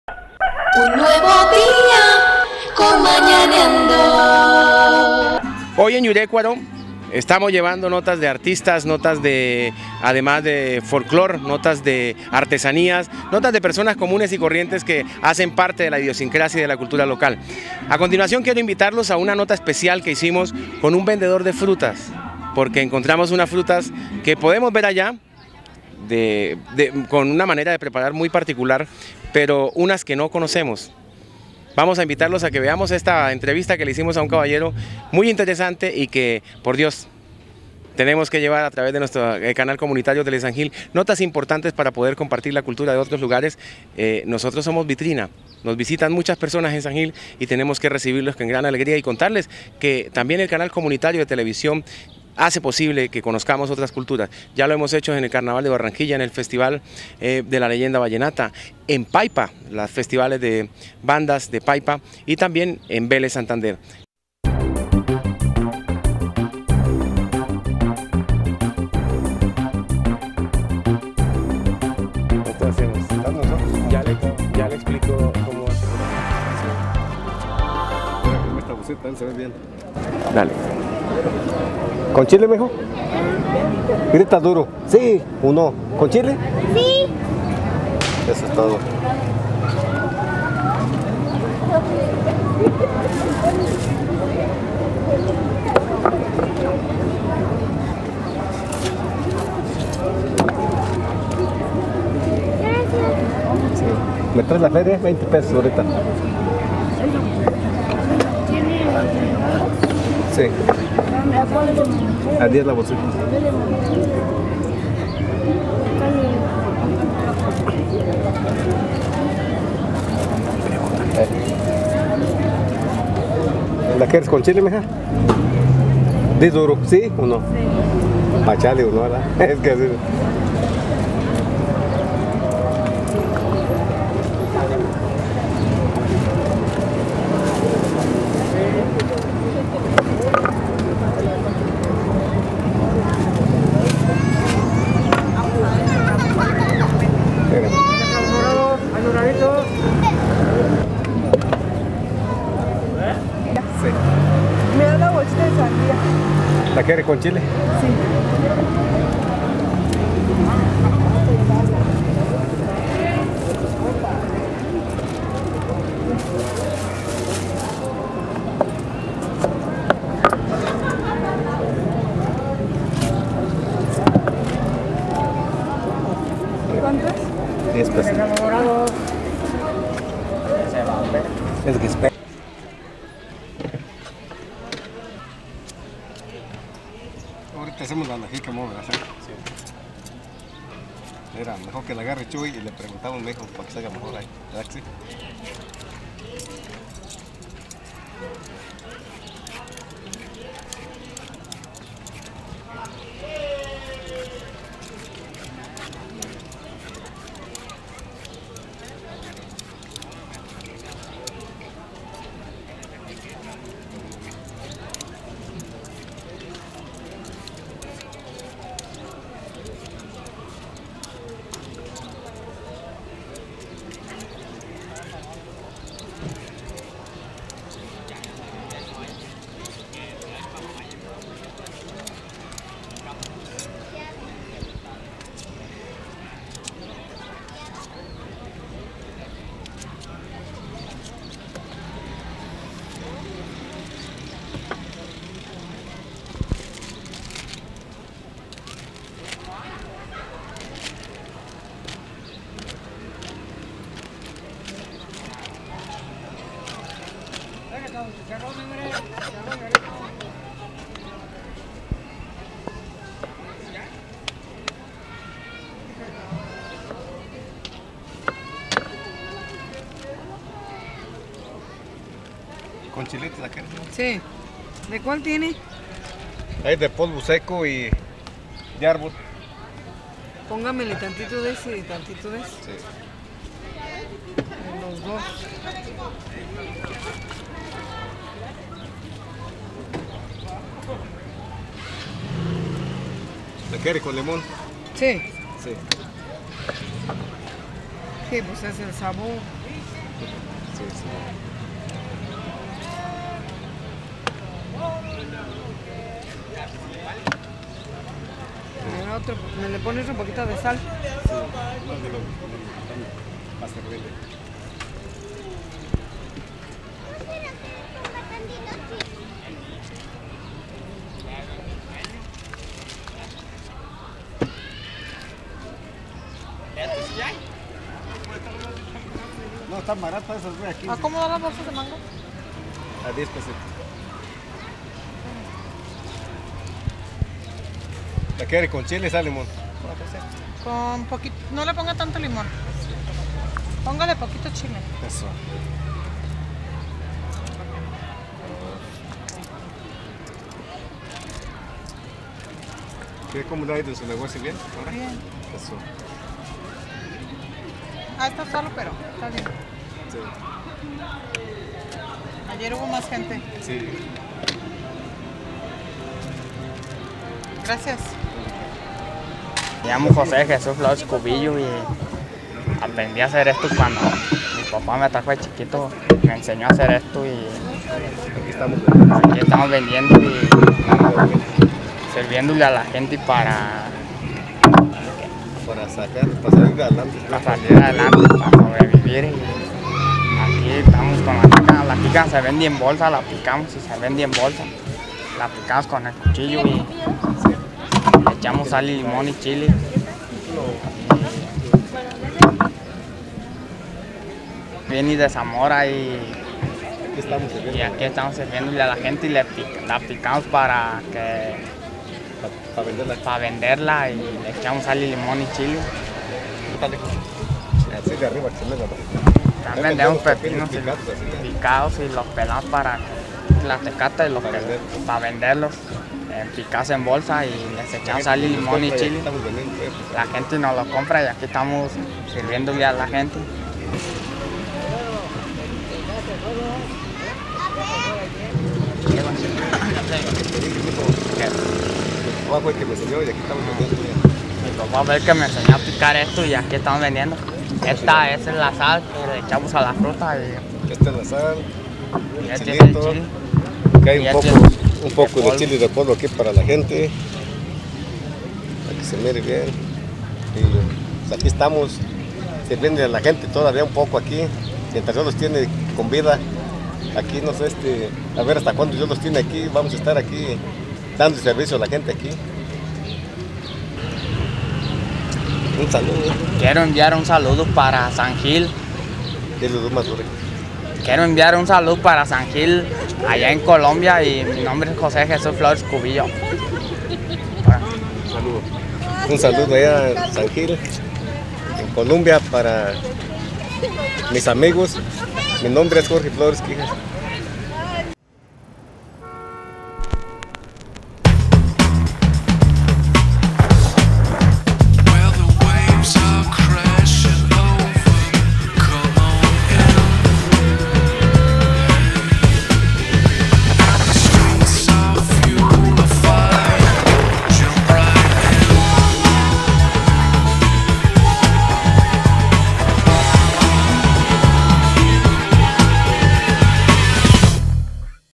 DELICIAS MEXICANAS
Josè de Jesùs Florez Cubillos y Jorge Florez, dos mexicanos vendedores de frutas tropicales en la plaza principal de Yurècuaro en Michoacàn.  Provòquese con el chile, el limón , la sal y las frutas propias de la regiòn.